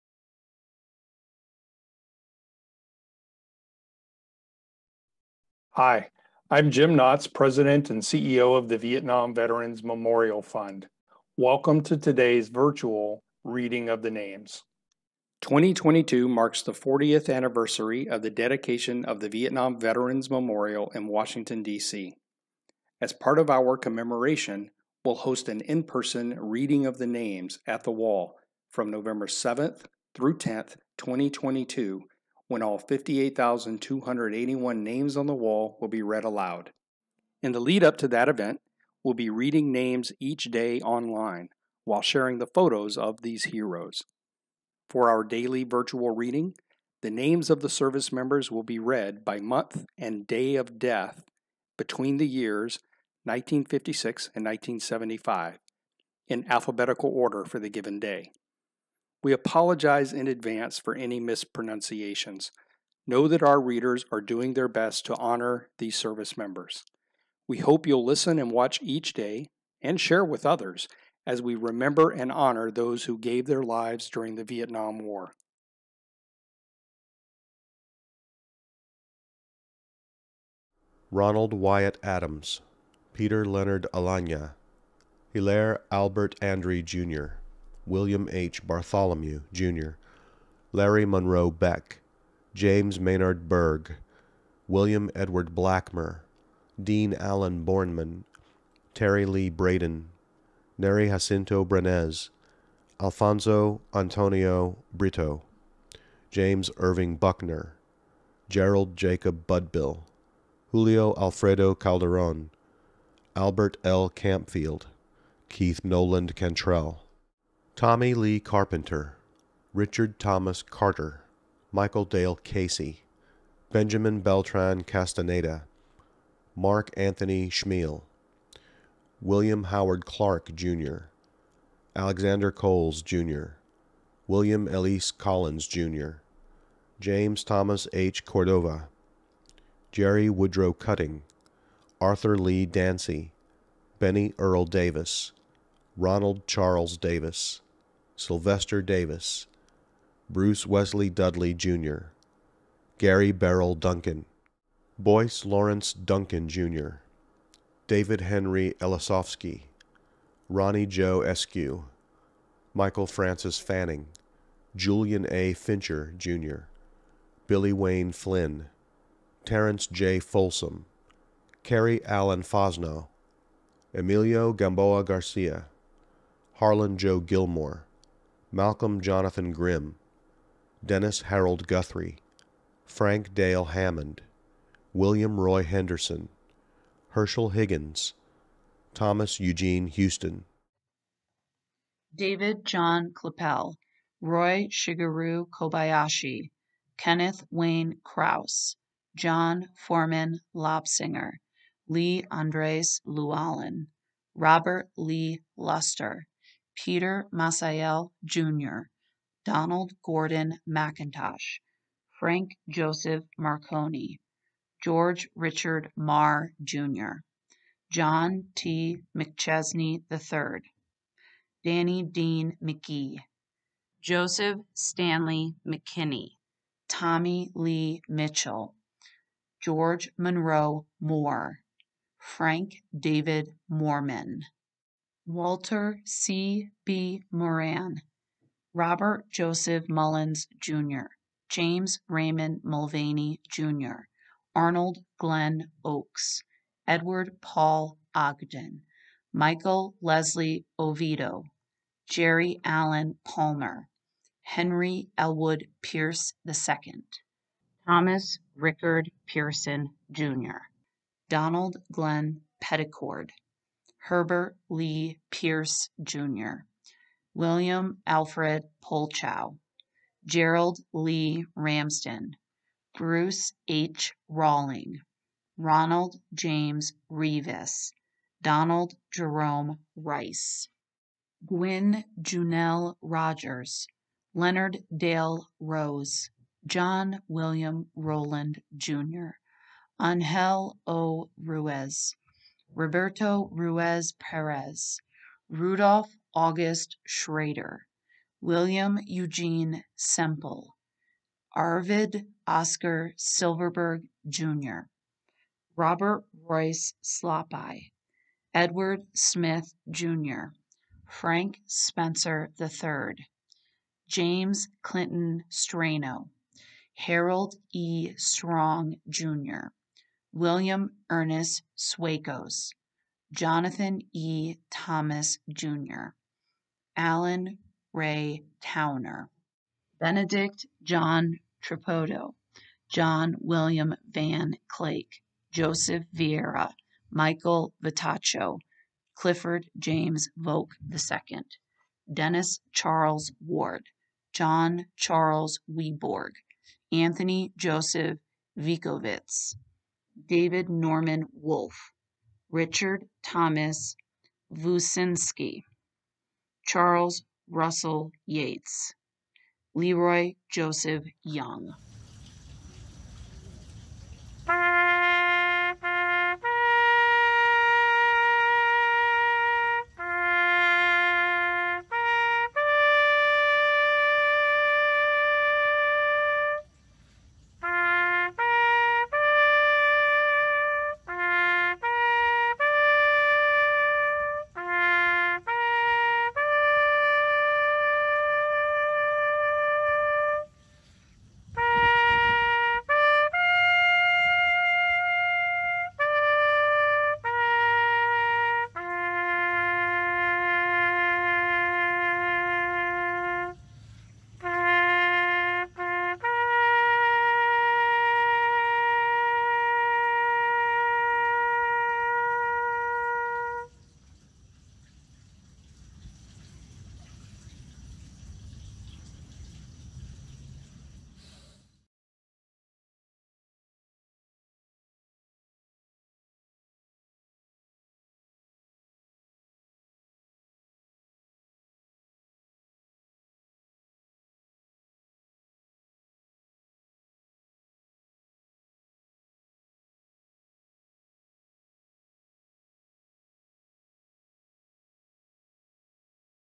Daily Virtual Reading of the Names: January 23
We honor those that died on January 23 between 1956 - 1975 during the Vietnam War. This series is a lead-up to our in-person Reading of the Names event at the Vietnam Veterans Memorial in Washington, DC between November 7-10, 2022.